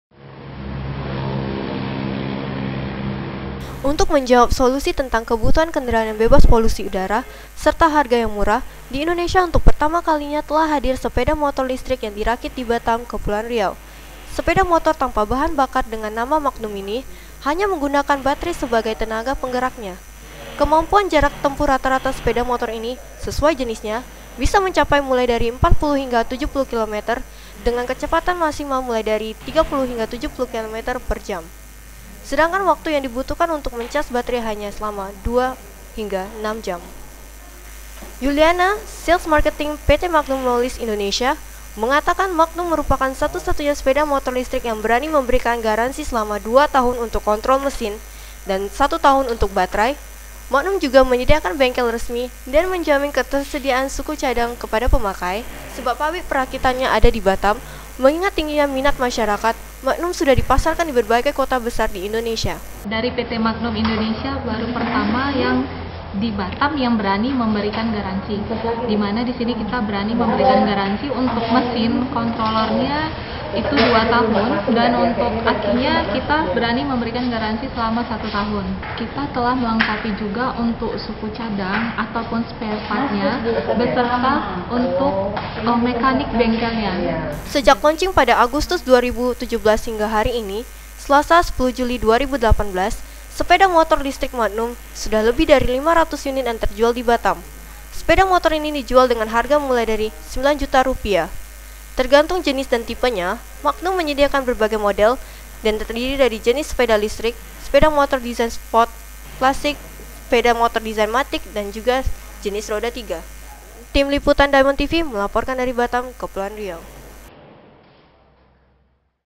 Motor Listrik Bebas Polusi Pertama Dirakit di Batam
DiamondOneNews, Batam - Untuk menjawab solusi tentang kebutuhan kendaraan yang bebas polusi udara, serta harga yang murah di Indonesia untuk pertama kalinya hadir sepeda motor listrik yang dirakit di Batam, Kepulauan Riau. Sepeda motor tanpa bahan bakar dengan nama magnum ini, hanya menggunakan baterai sebagai tenaga penggeraknya. Kemampuan jarak tempuh rata rata sepeda motor ini, sesuai jenisnya, bisa mencapai mulai dari 40 hingga 70 kilometer per jam, sedangkan waktu yang dibutuhkan untuk mencas baterainya hanya selama 2 hingga 6 jam.
Yuliana, sales marketing PT Magnum Molis Indonesia mengatakan, Magnum merupakan satu-satunya sepeda motor listrik yang berani memberikan garansi selama dua tahun untuk kontrol mesin, dan satu tahun untuk baterai. Magnum juga menyediakan bengkel resmi dan menjamin ketersediaan suku cadang kepada pemakai, sebab pabrik perakitannya ada di Batam, mengingat tingginya minat masyarakat Magnum sudah dipasarkan di berbagai kota besar di Indonesia.
Sejak launching Agustus 2017 hingga hari ini, selasa (10/7/2018) sepeda motor listrik Magnum, sudah lebih dari 500 unit yang terjual di Batam. Sepeda motor ini dijual dengan harga mulai dari 9 juta rupiah, tergantung jenis dan tipenya, Magnum menyediakan berbagai model dan terdiri dari jenis sepeda listrik, sepeda motor desain sport, klasik, sepeda motor desain matic dan jenis roda tiga.

(red)